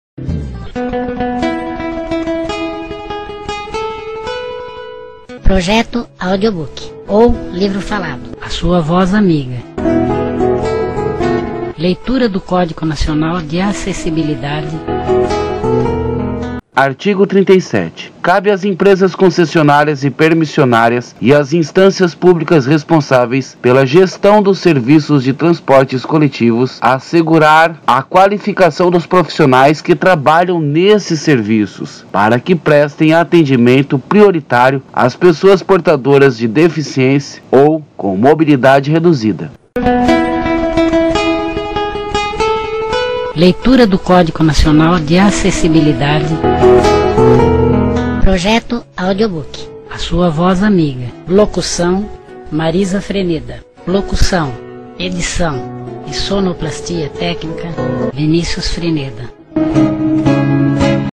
LEITURA. DO. DECRETO 5296 ARTIGO 37 CÓDIGO NACIONAL DE ACESSIBILIDADE LETRAS GARRAFAIS
LOCUÇÃO  :  VINÍCIUS   FRENEDA  .
LOCUÇÃO DE  APOIO  :  MARÍZA   FRENEDA  .
PROJETO   ÁUDIO  BOOK  Á  SUA  VÓZ  AMIGA  NO  YOU-TUBE  BRASIL .
EDIÇÃO  DE  ÁUDIO E  LETRASS  GARRAFAIS : VINÍCIUS FRENEDA  .
IMAGENS : GOOGLE  IMAGENS  .